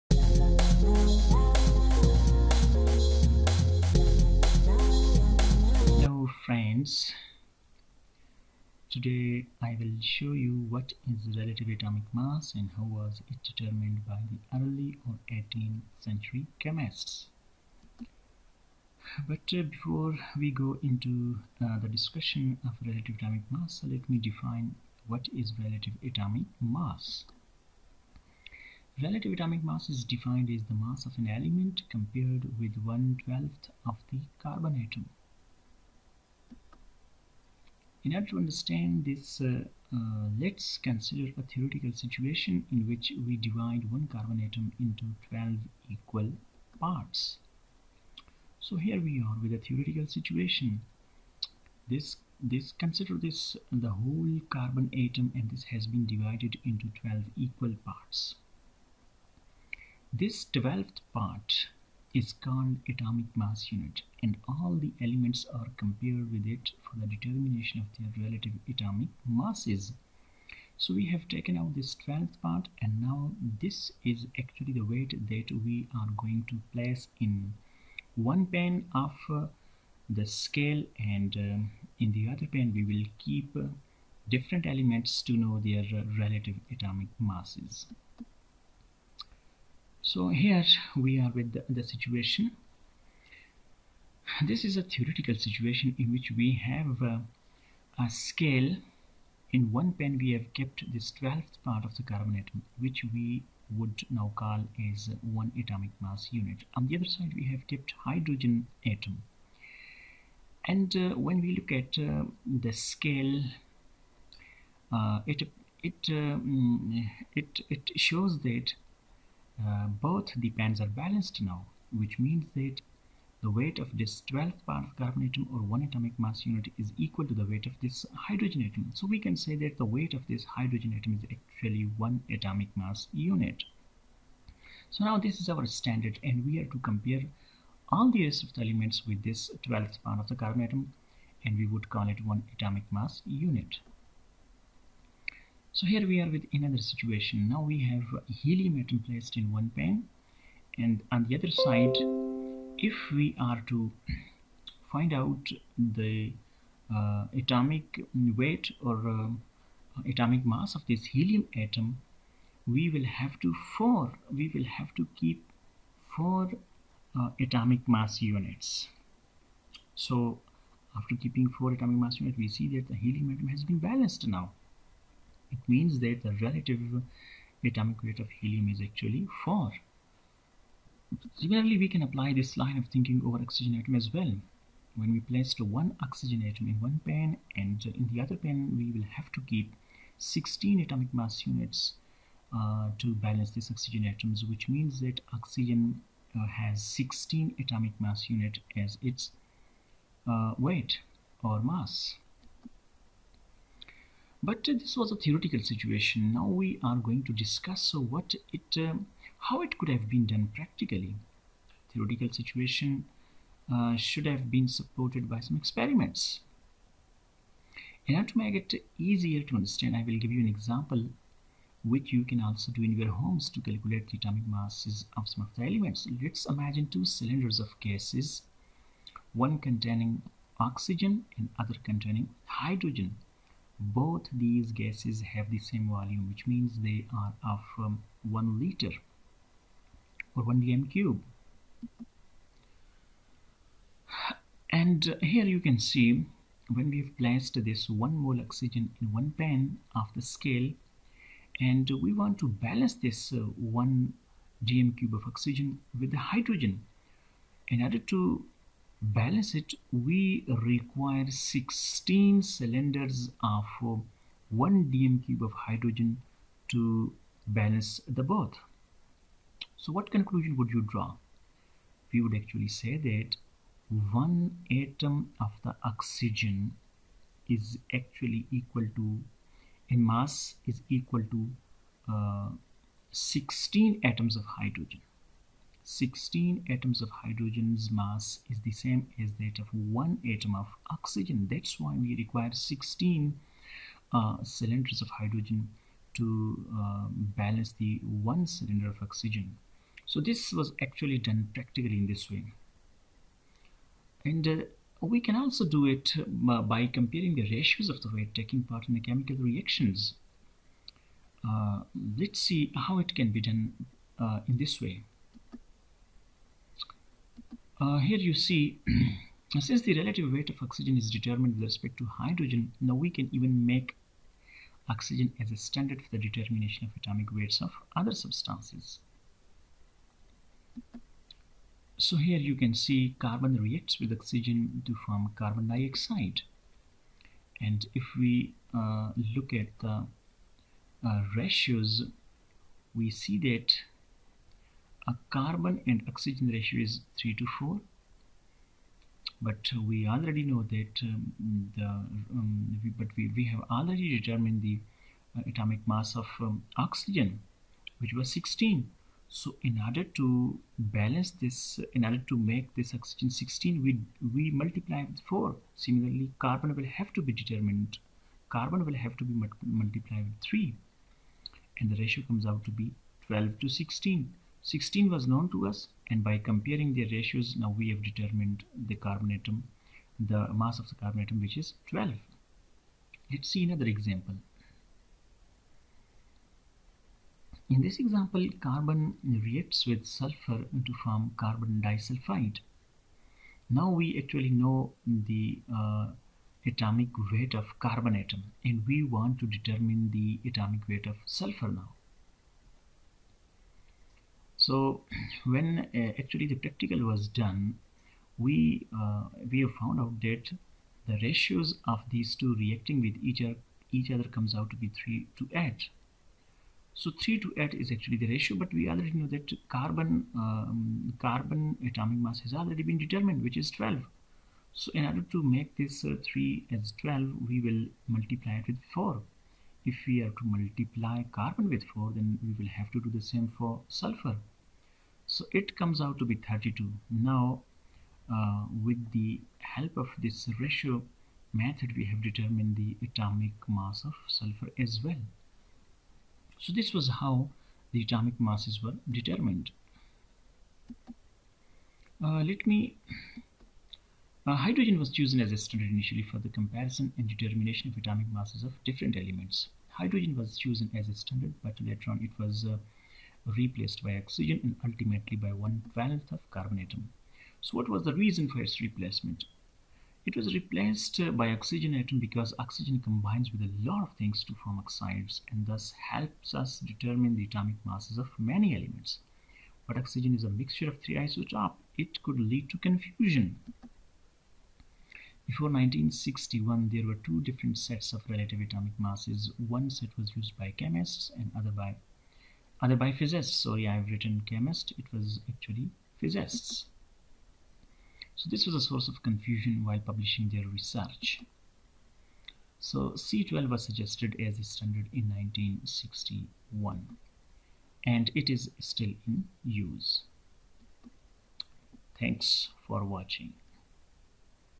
What is Relative Atomic Mass |How was Relative Atomic Mass calculated first
In this video, we explore the concept of relative atomic mass. It is defined as the mass of an element compared to 1/12th of the mass of a carbon atom with a mass number of 12 (C-12). Initially, hydrogen was chosen as the standard for the comparison and determination of atomic masses of different elements. However, it was later replaced by the oxygen atom. This is because oxygen combines with a lot of elements to form oxides and helps determine the atomic masses of many elements. But since oxygen is a mixture of three isotopes, it could lead to confusion and fractional atomic masses.

Before 1961, there were two different sets of relative atomic masses used by chemists and physicists, causing confusion when publishing research. In 1961, C-12 was suggested as the standard and is still in use today.